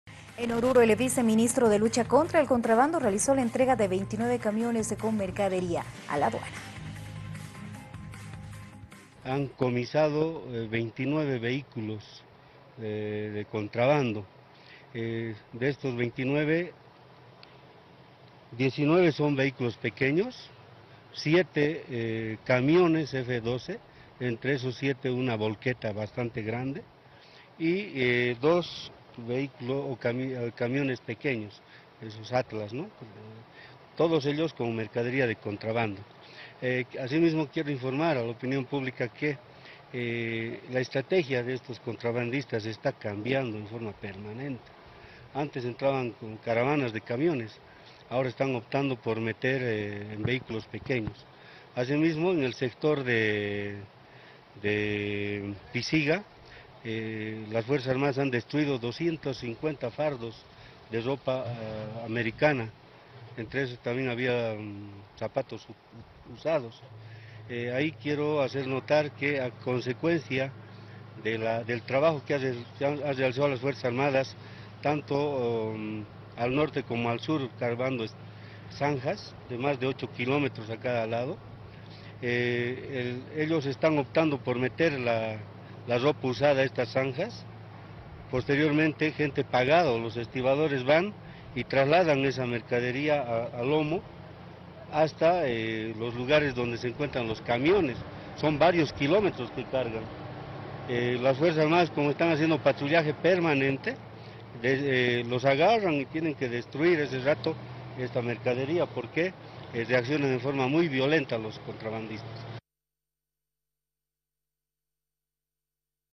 CEO secuestra 29 vehículos con mercadería de contrabando en Oruro
Gonzalo Rodríguez, viceministro de Lucha Contra el Contrabando, informó que efectivos del Comando Estratégico Operacional (CEO) secuestraron 19 vehículos pequeños, siete camiones F 12, una volqueta bastante grande y otros dos camiones pequeños, con mercadería de contrabando en Oruro, los cuales fueron entregados a dependencias de la Aduana Nacional para realizar los trabajos correspondientes en la requisa de la mercadería.